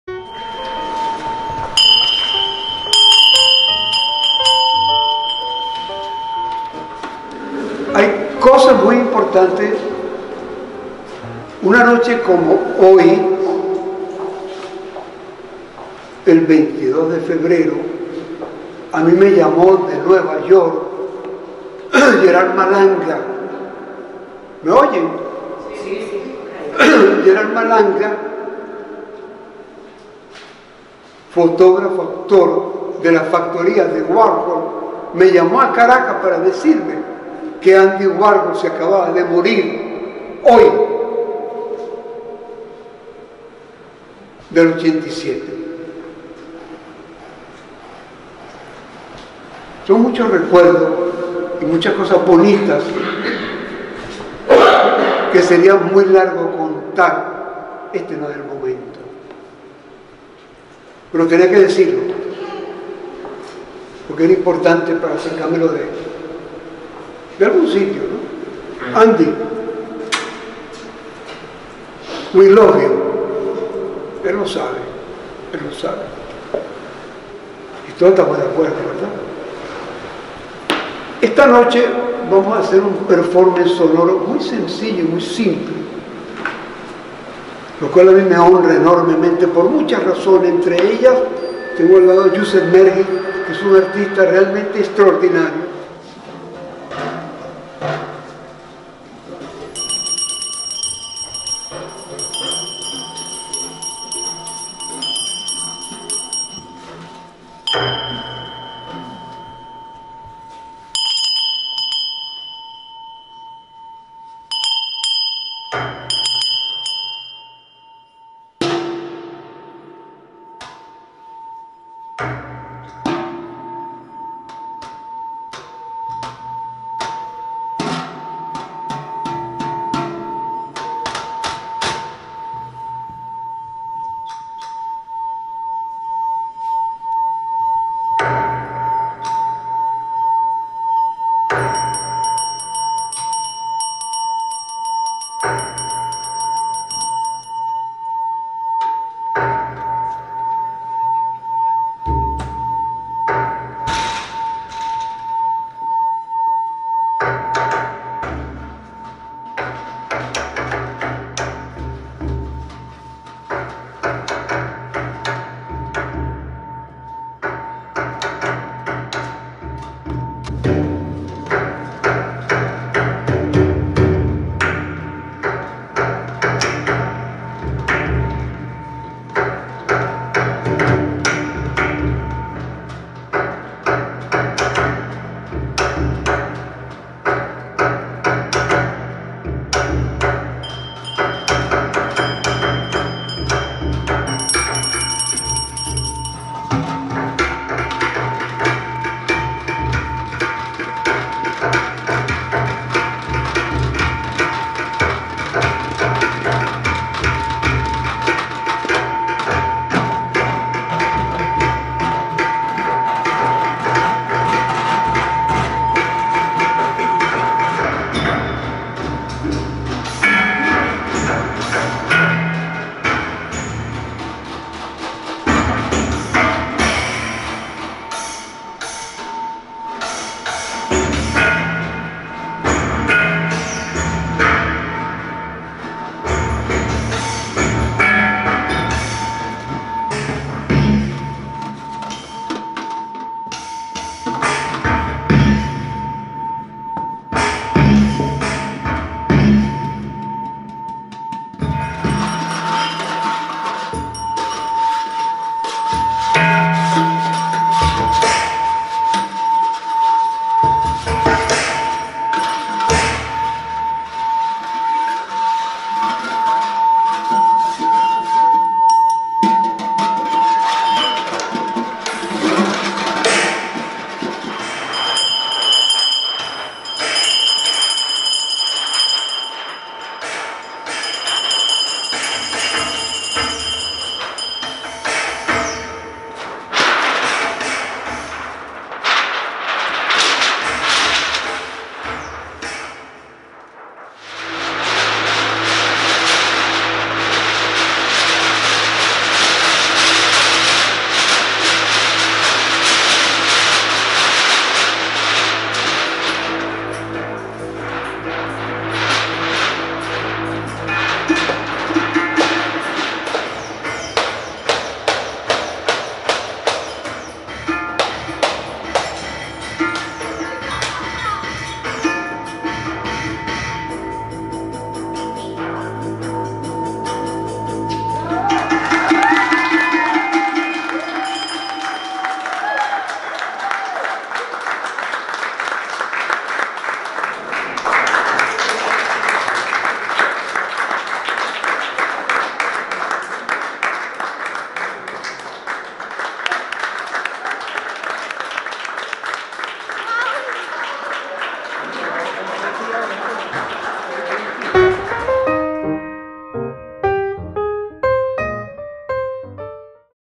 Rolando Peña Performance Behind my Eyes
Behind my Eyes by Rolando Peña with Andres Michelena and Yucef Merhi
From
A Matter of Perspective by Rolando Peña
About the Current Art Exhibition, A Matter of Perspective photographs by Rolando Peña ArtMedia Gallery is pleased to present Rolando Peña's exhibition A Matter of Perspective, concentrated on a segment of the artist's varied artistic production from the first decades of his trajectory, the 60s and 70s of the XX century.

Cuestión de perspectiva de Rolando Peña.
Sobre nuestra Exposición de Arte Actual, Cuestión de Perspectiva fotografías de Rolando Peña ArtMedia Gallery se complace en presentar la exposición de Rolando Peña Cuestión de Perspectiva, concentrada en un segmento de la variada producción artística del artista desde las primeras décadas de su trayectoria, las décadas del 60 y 70 del siglo XX.